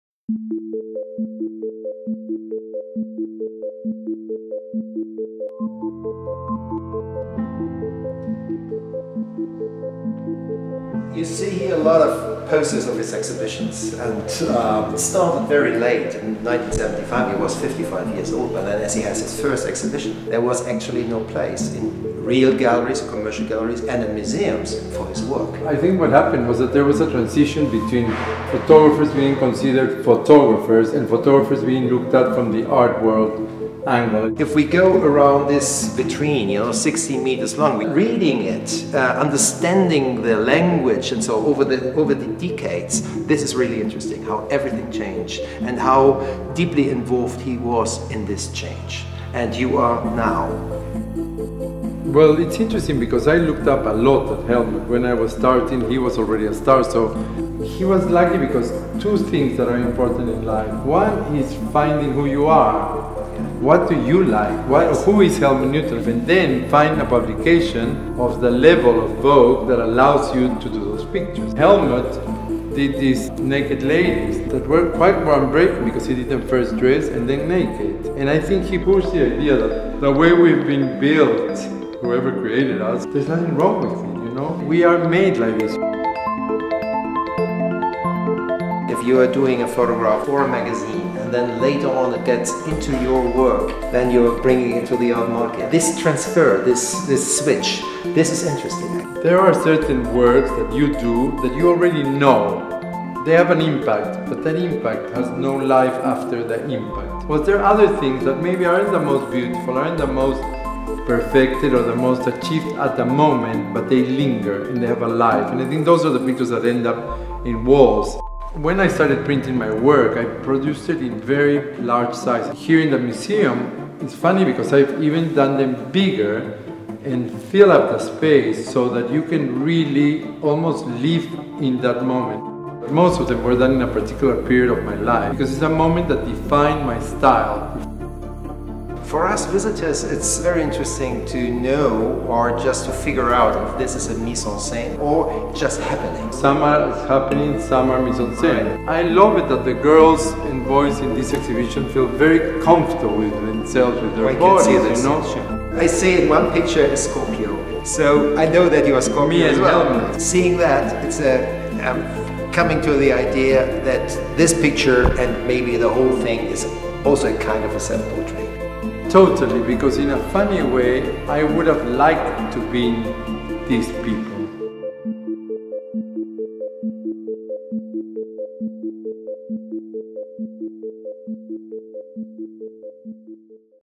Framing History: Mario Testino and Matthias Harder at the Helmut Newton Foundation
Every artist has their idols. Helmut Newton is among Mario Testino’s, his inimitable style and stylized nudity shaping a generation. Joined by Curator of the Helmut Newton Foundation, Matthias Harder, the pair walk through the landscape of bodies on display. Discussing the striking Helmut Newton archive, and the longevity of his influence, they put the history of photography in perspective.